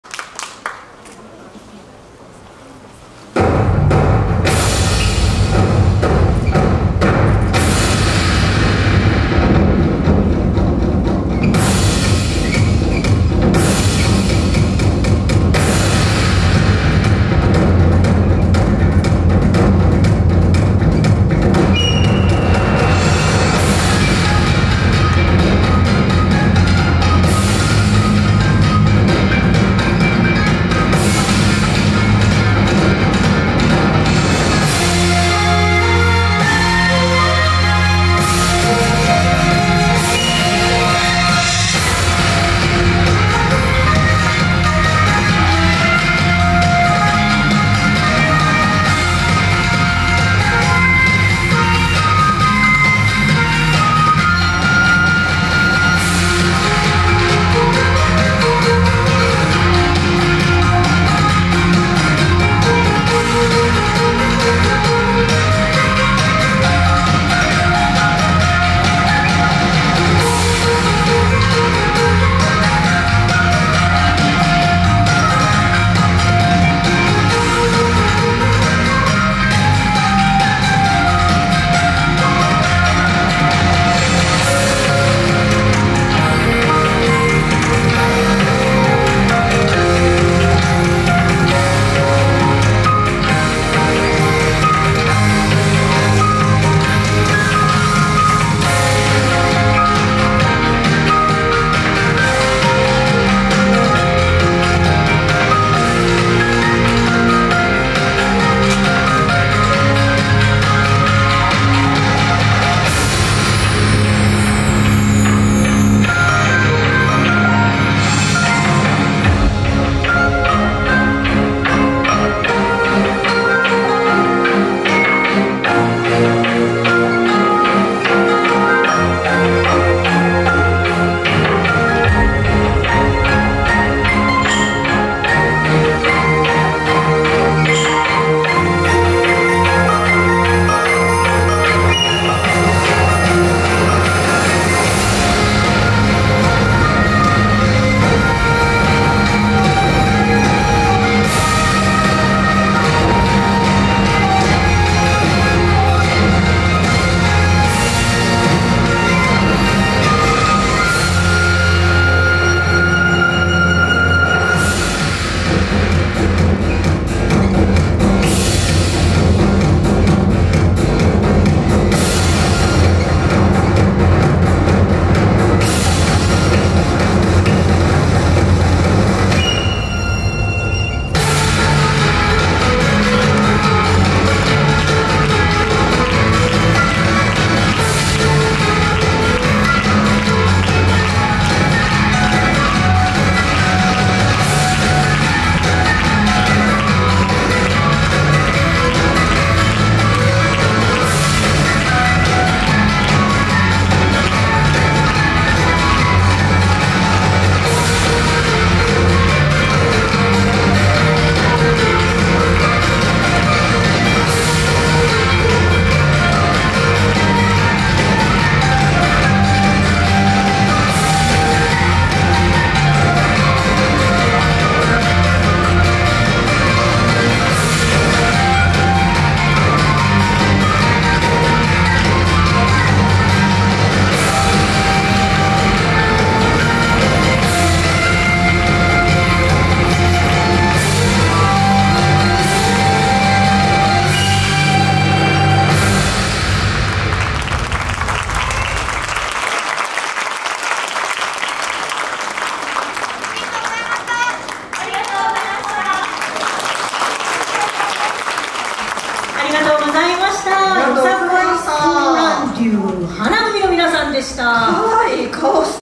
よさこいチーム龍 華組 @ よさこい大阪大会 (イエスシアター/第2部) 2021-11-13T13:41
2021年11月13日(土)、大阪府大阪市で開催された「よさこい大阪大会」(イエスシアター/第2部)での「よさこいチーム龍 華組」さんの演舞です。